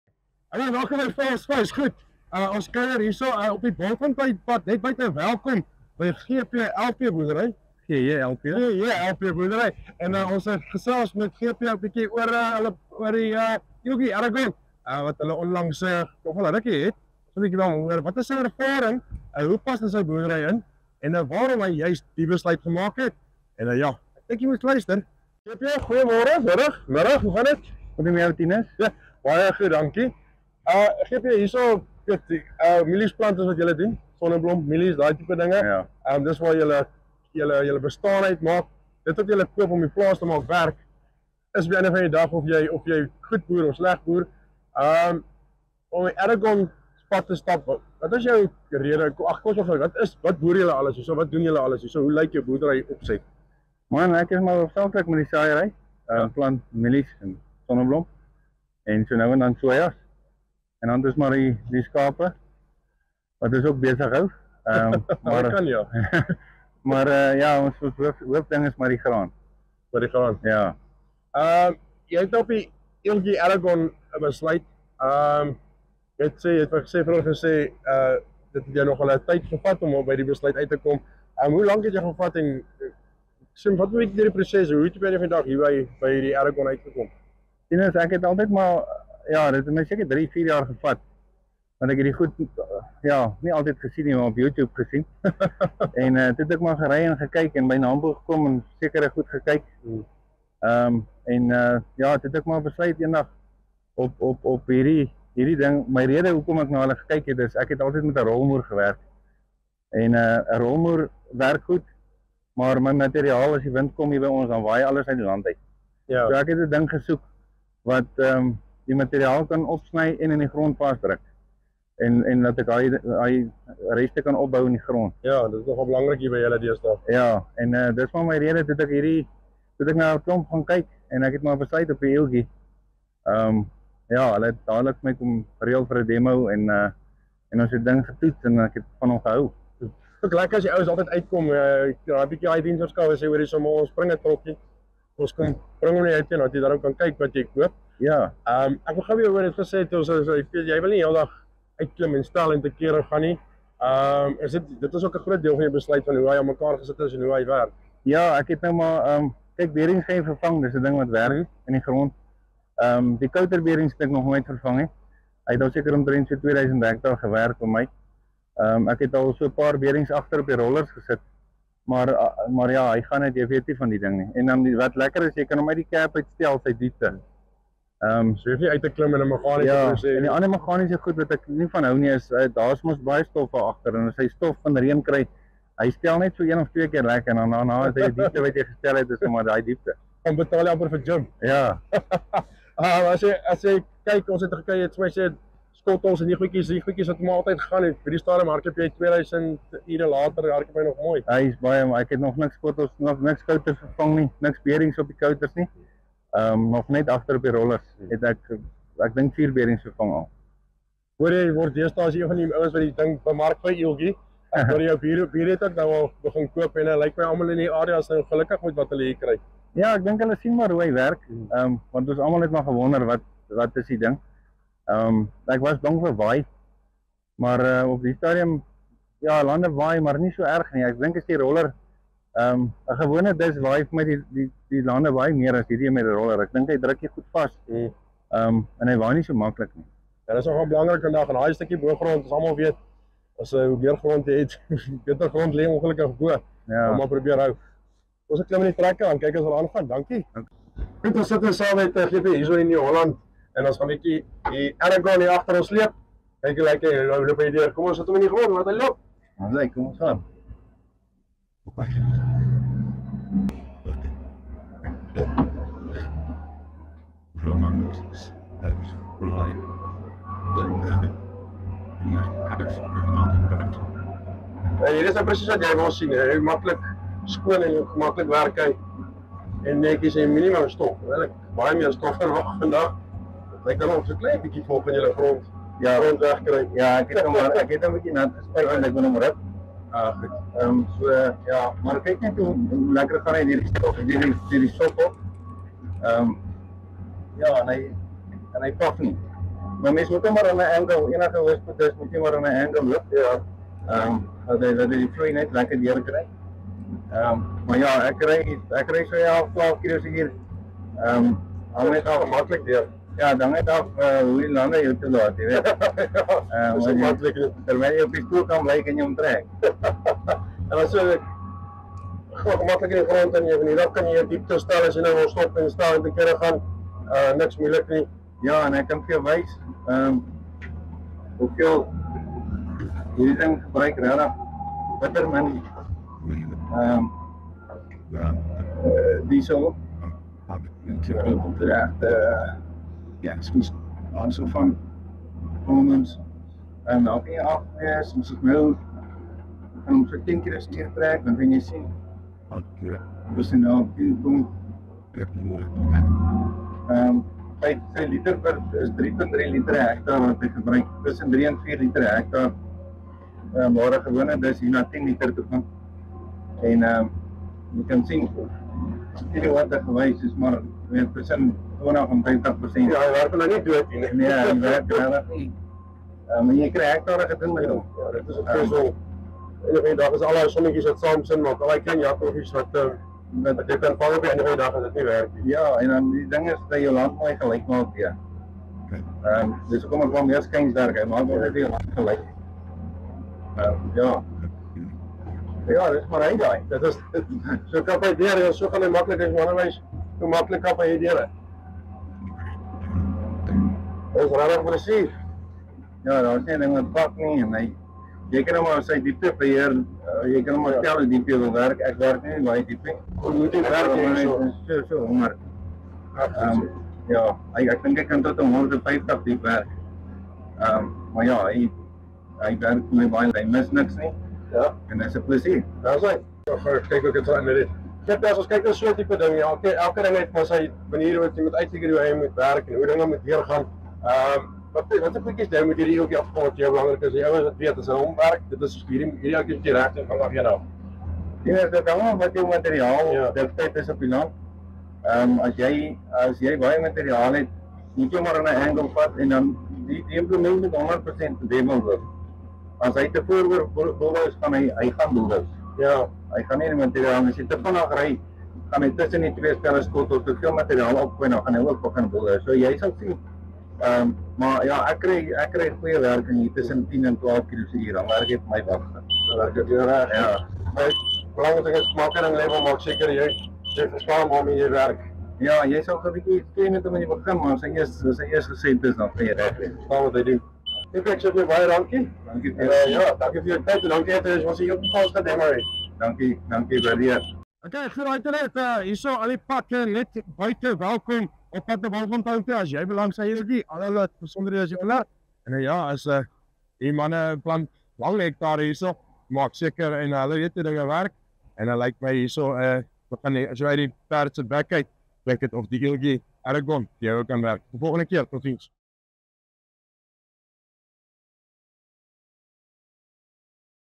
GP van GJLP Boerdery net buite Welkom vertel hoekom jy die ILGI Aragon op jou plaas moet gebruik.
GP van die GJLP Boerdery net buite Welkom vertel ons hoekom jy die ILGI Aragon op jou plaas moet gebruik. GP gebruik sy ILGI Aragon al n rukkie. Wat is sy ervaring en hoekom het hy die besluit gemaak?